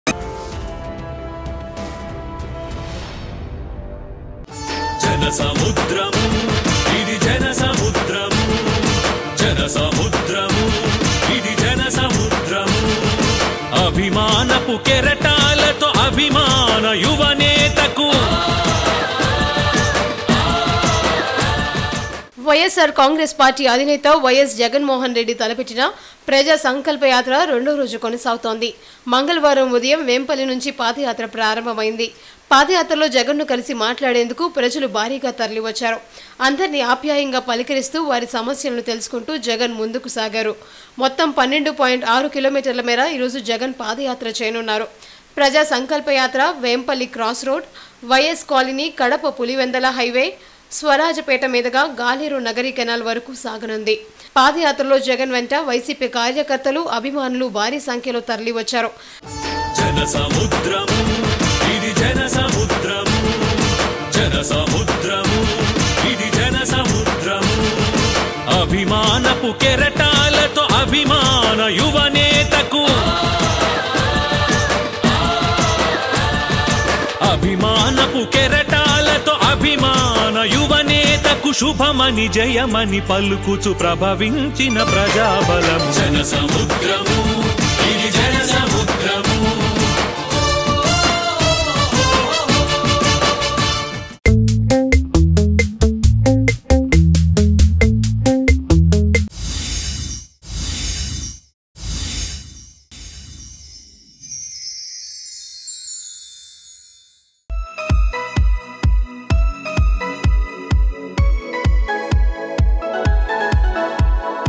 AMPM Live : YS Jagan starts 2nd Day of Praja Sankalpa Padayatra
AMPM Live : వైఎస్సార్‌ కాంగ్రెస్‌ పార్టీ అధినేత వైఎస్‌ జగన్ మోహన్ రెడ్డి తలపెట్టిన ప్రజా సంకల్పయాత్ర రెండో రోజు కొనసాగుతోంది. మంగళవారం ఉదయం వేంపల్లి నుంచి పాదయాత్ర ప్రారంభమయ్యింది. పాదయాత్రలో జగన్ ను కలిసి మాట్లాడేందుకు ప్రజలు భారీగా తరలివచ్చారు. అందరినీ ఆప్యాయంగా పలకరిస్తూ, వారి సమస్యలు తెలుసుకుంటూ జగన్ ముందుసాగారు. మొత్తం 12.6 కిలోమీటర్ల మేర ఈ రోజు జగన్ పాదయాత్ర చేయనున్నారు. ప్రజా సంకల్పత్ర వేంపల్లి క్రాస్‌ రోడ్డు, వైఎస్‌.కాలనీ, కడప-పులివెందుల హైవే, సర్వరాజపేట మీదుగా గాలేరు-నగరి కెనాల్‌ వరకూ సాగనుంది. రాత్రి 8.30 గంటలకు ప్రొద్దుటూరు రోడ్డులోని నేల తిమ్మాయపల్లి వద్ద ఏర్పాటు చేసిన బసకు జగన్ చేరుకుంటారు.